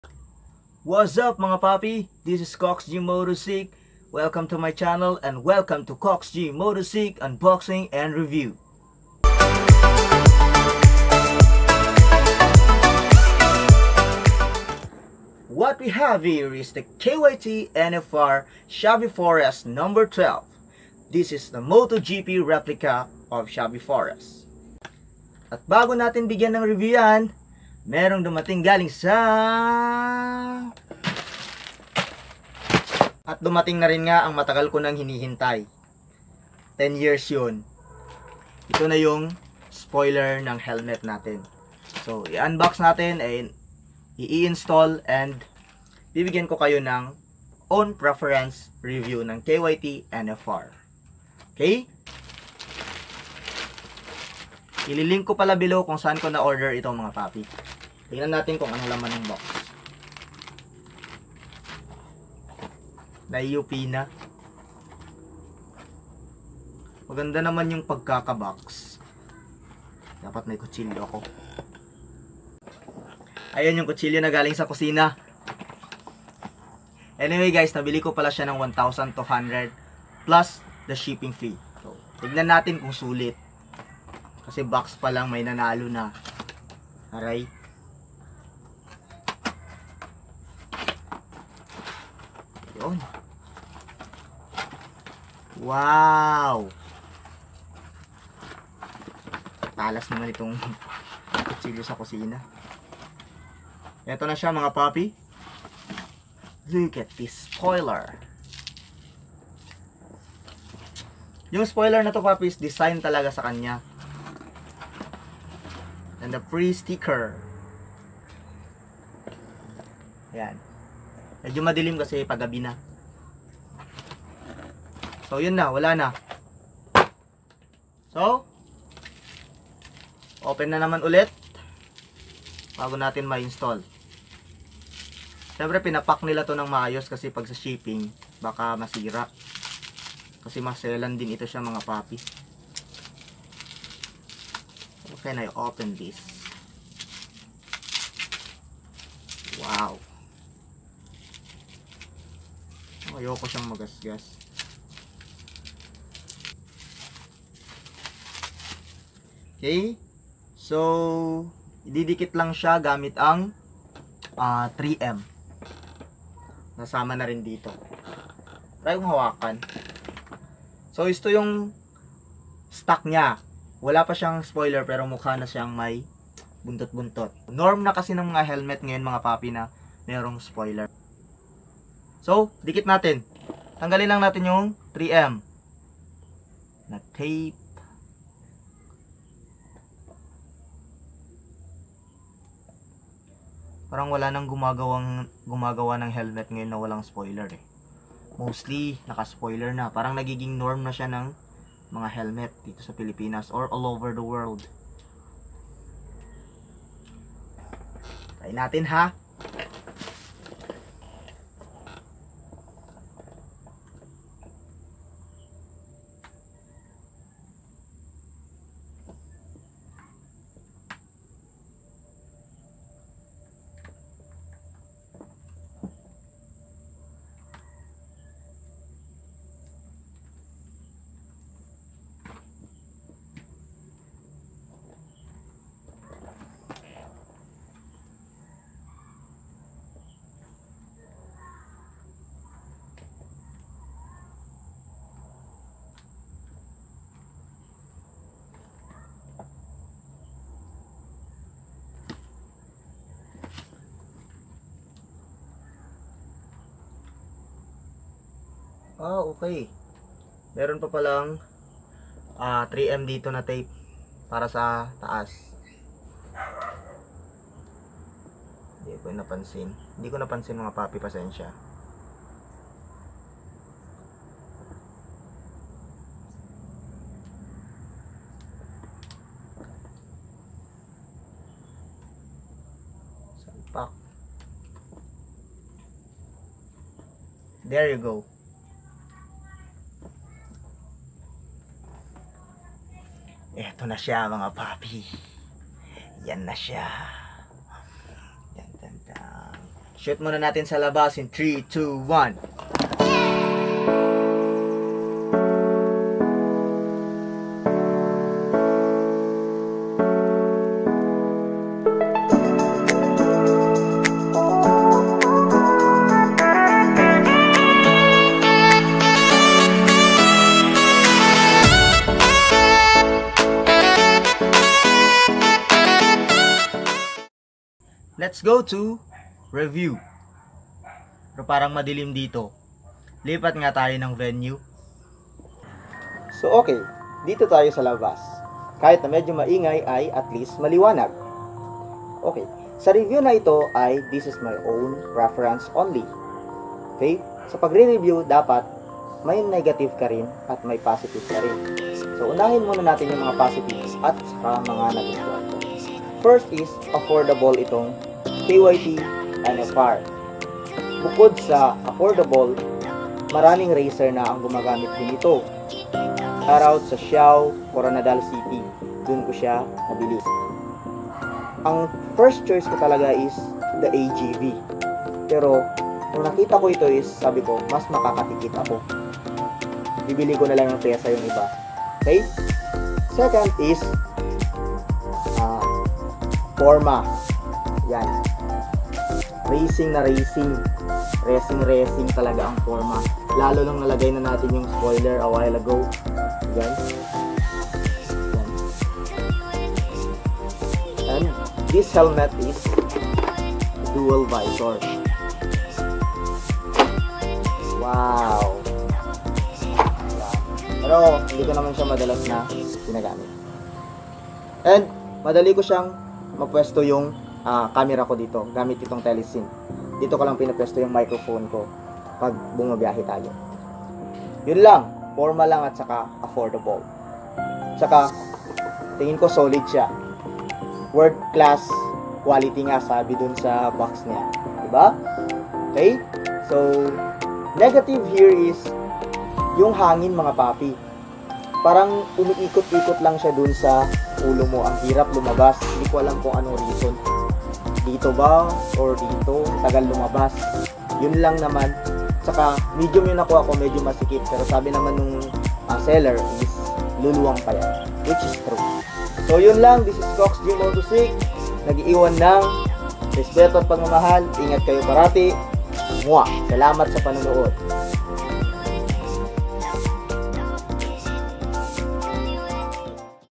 Kyt NFR Xavi Fores review (spoiler unboxing and install )
Quick review (own preference) on KYT NFR HELMET
and install its spoiler.